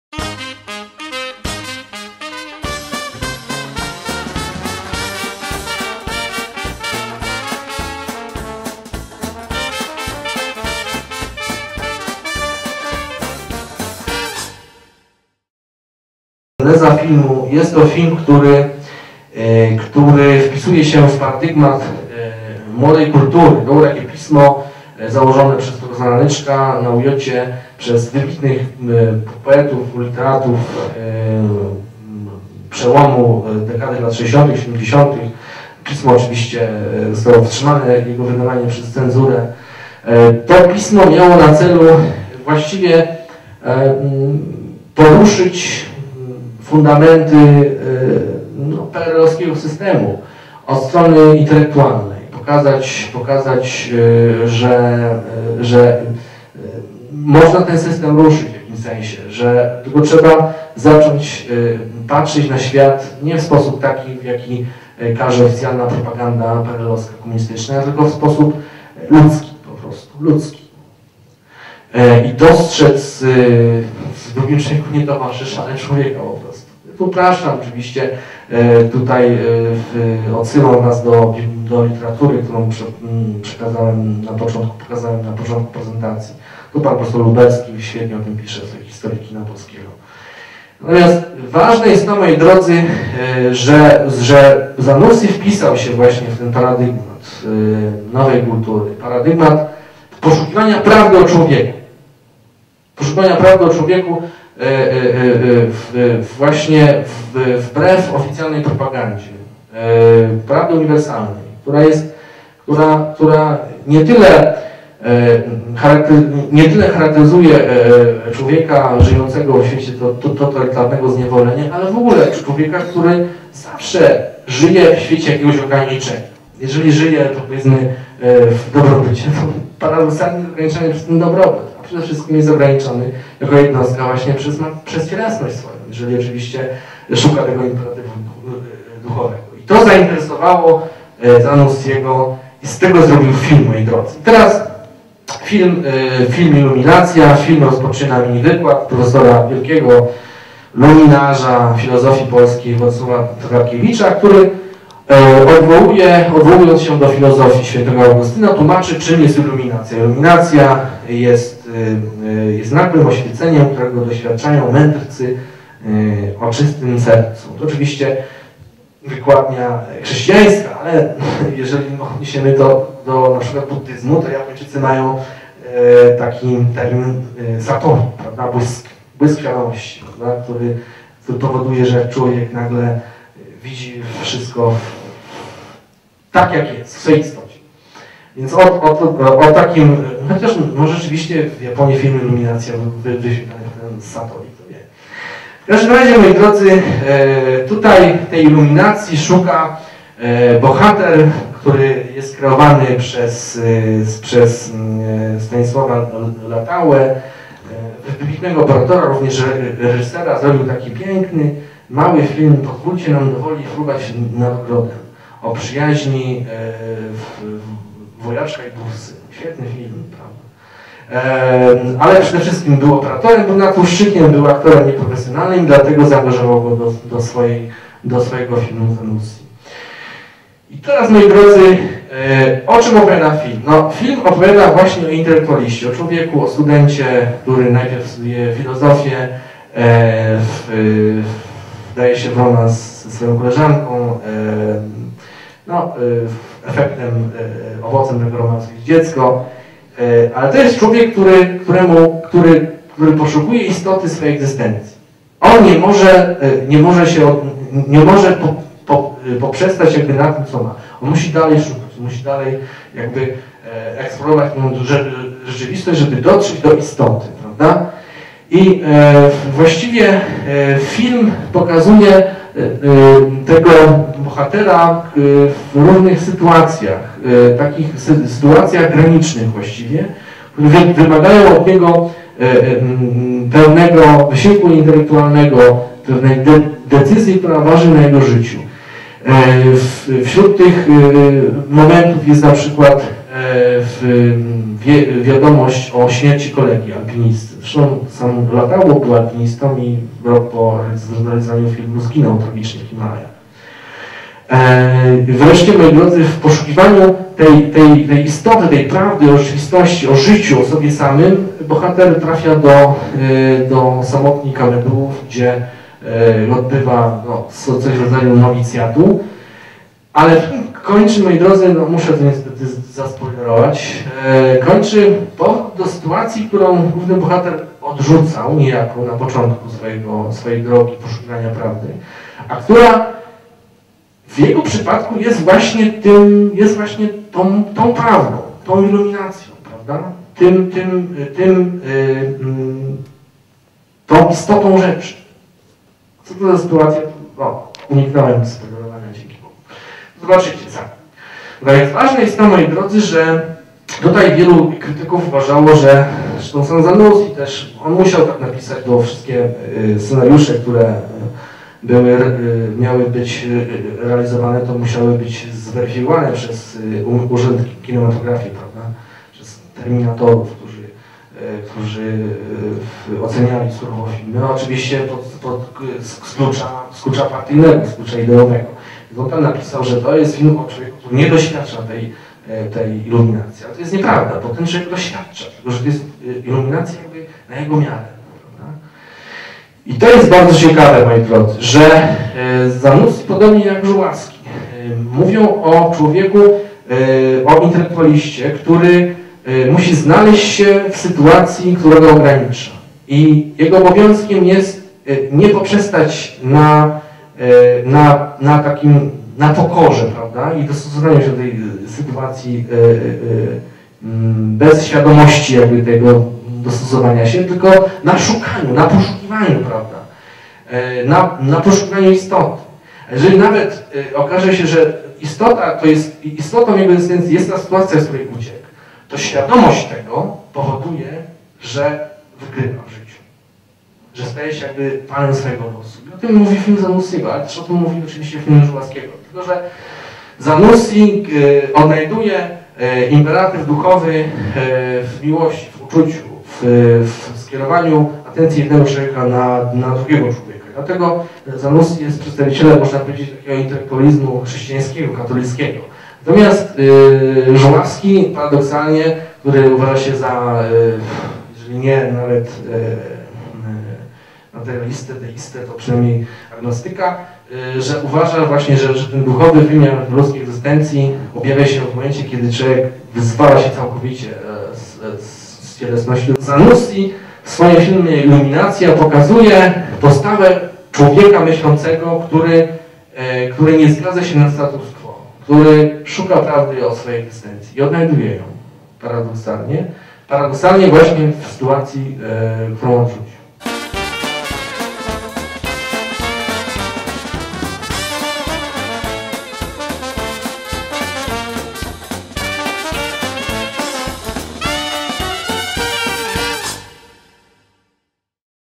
ILUMINACJA, dr Piotr Kletowski
Dr Piotr Kletowski o filmie "Iluminacja" w reżyserii Krzysztofa Zanussiego.
Wykład wygłoszony na Akademii Polskiego Filmu 21 lutego 2011 r. w Łodzi.